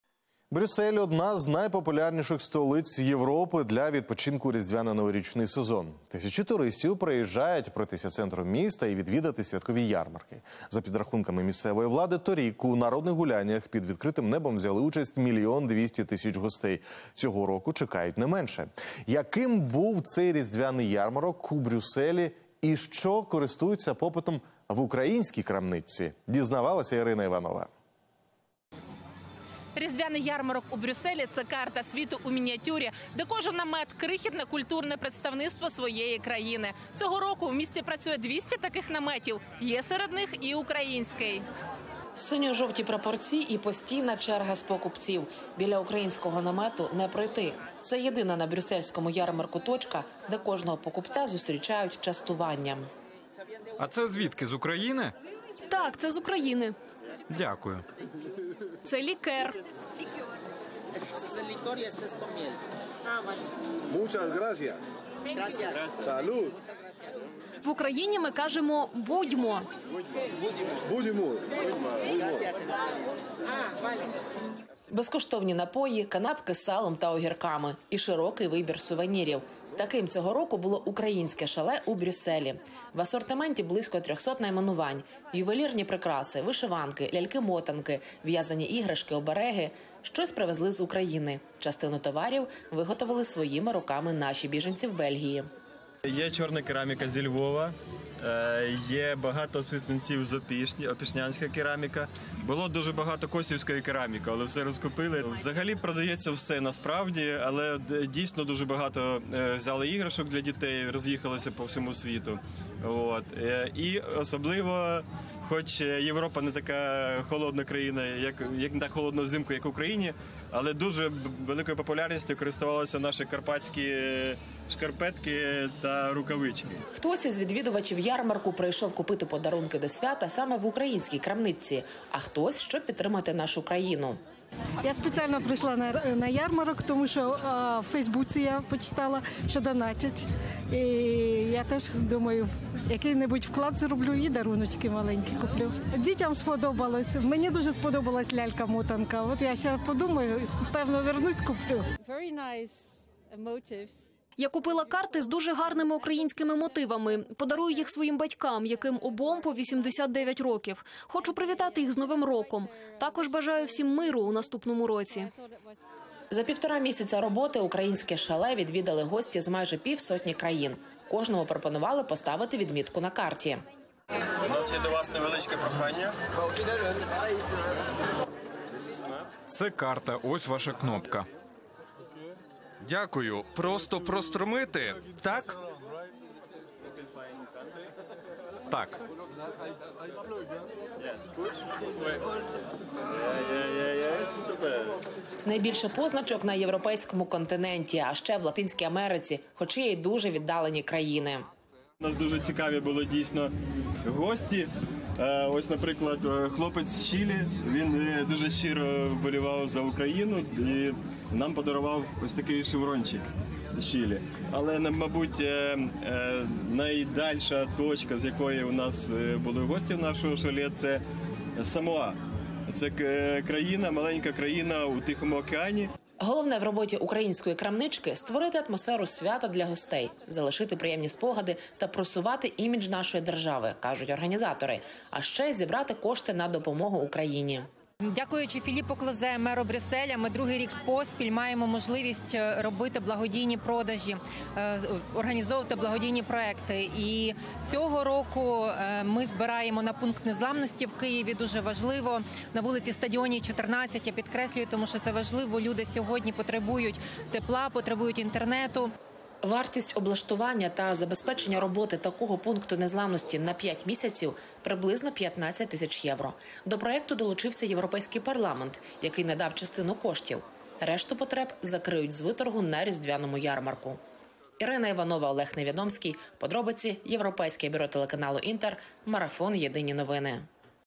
У Брюсселі відкрили українську крамничку на різдвяному ярмарку
Брюссель - одна з найпопулярніших столиць Європи для відпочинку у різдвяно-новорічний сезон. Яким був цей різдвяний ярмарок у Брюсселі, і що має попит в українській крамничці?